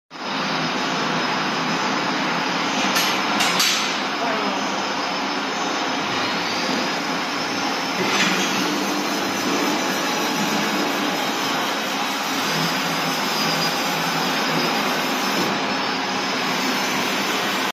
2000L Brewery equipment auto lift up and down grain rake
20HL brewery equipment auto lift up and down grain rake.
Emily
Tiantai beer equipment company